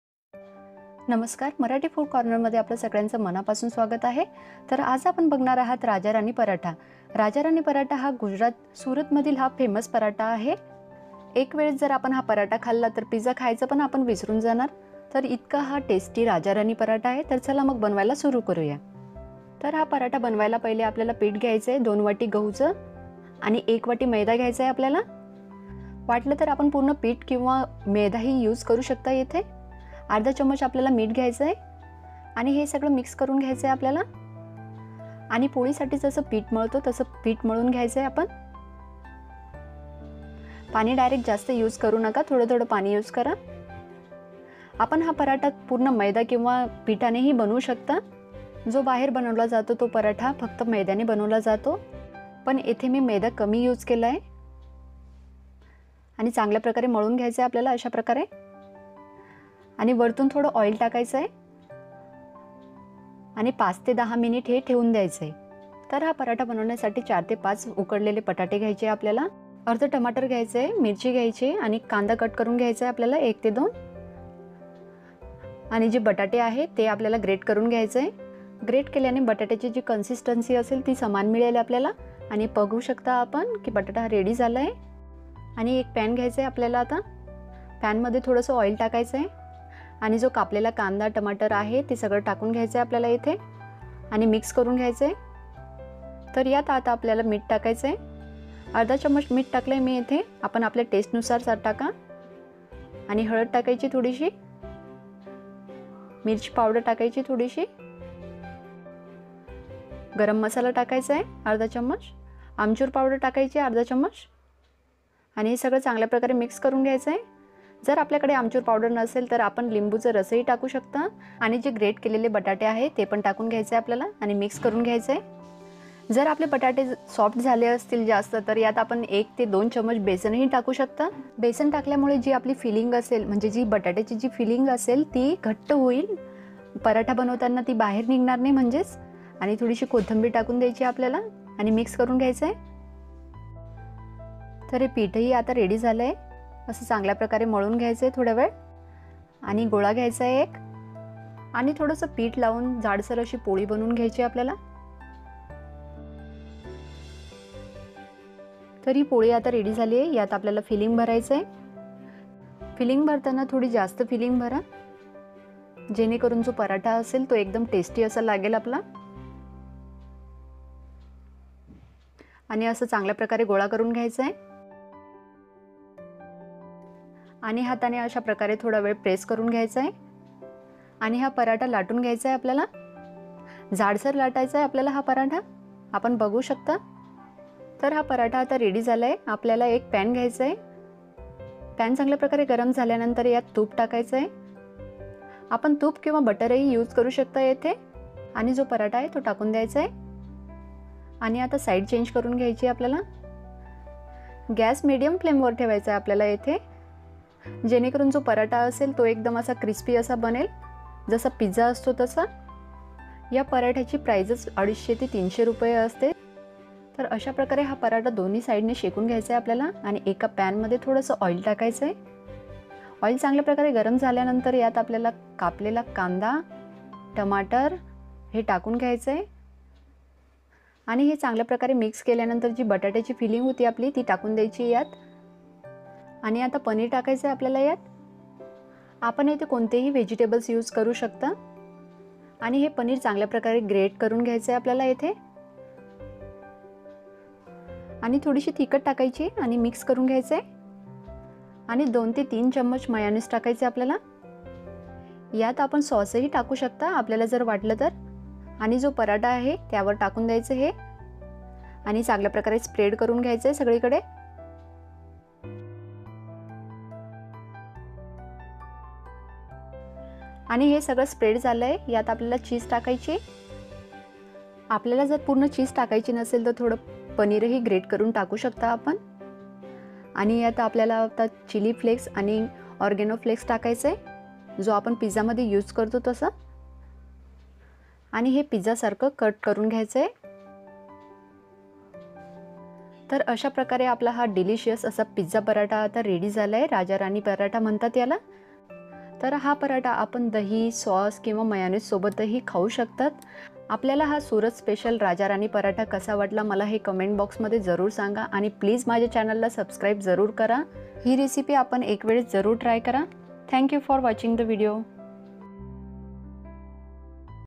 Aloo Paratha तर खूप खालं असेल असा पराठा खाणार तर pizza विसरणार/ pizza paratha | zatpat nashta recipe
Aloo Paratha तर खूप खालं असेल असा पराठा खाणार तर pizza विसरणार/ pizza paratha | zatpat nashta recipe (@MarathiFoodCorner )